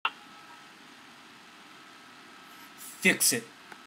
Jonny Lam, Artist supports an OnionPulitzer
The frustration is mounting in everyday Americans. This one from Jonny Lam in Portland uncorks several decades of building frustration.